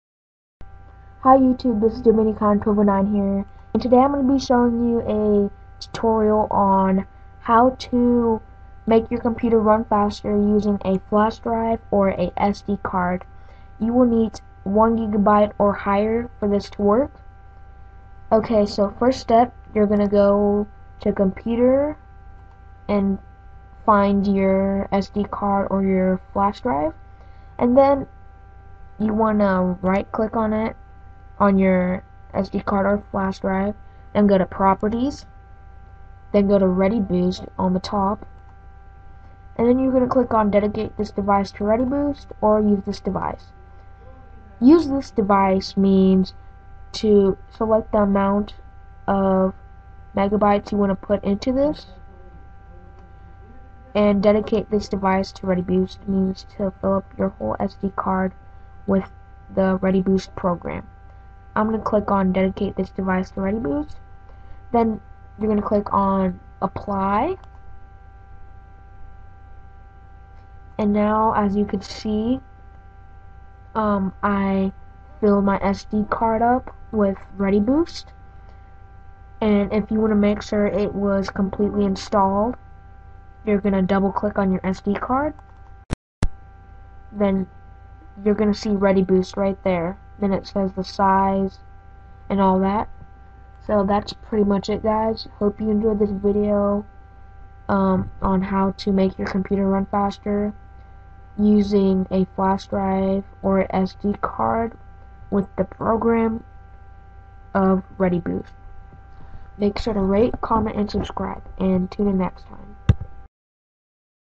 How to make your computer run faster, using a sd card or flashdrive(readyboost)-Part 2
Go to start then computer then right click your sd card or flashdrive then go to properties then readyboost on the top then click on dedicate this device or use this device then click apply.